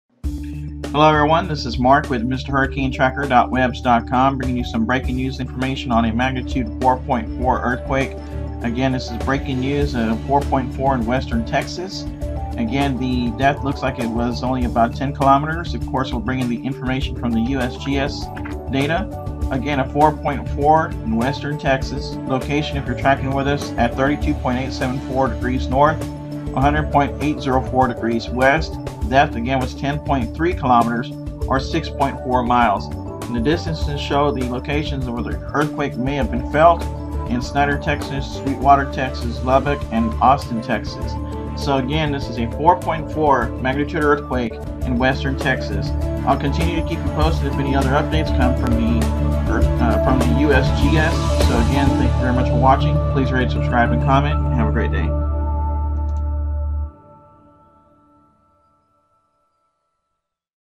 Earthquake update Magnitude 4.4 Western Texas
Magnitude 4.4
Date-Time Sunday, September 11, 2011 at 12:27:45 UTC
Sunday, September 11, 2011 at 07:27:45 AM at epicenter
Time of Earthquake in other Time Zones

Location 32.874°N, 100.804°W
Depth 10.3 km (6.4 miles)
Region WESTERN TEXAS
Distances 19 km (11 miles) NNE of Snyder, Texas
57 km (35 miles) NW of Sweetwater, Texas
126 km (78 miles) SE of Lubbock, Texas
409 km (254 miles) NW of AUSTIN, Texas

Location Uncertainty horizontal +/- 14.8 km (9.2 miles); depth +/- 7 km (4.3 miles)
Parameters NST=237, Nph=239, Dmin=112.4 km, Rmss=1.52 sec, Gp= 40°,
M-type=body wave magnitude (Mb), Version=6
Source Magnitude: USGS NEIC (WDCS-D)
Location: USGS NEIC (WDCS-D)

Event ID usc0005shp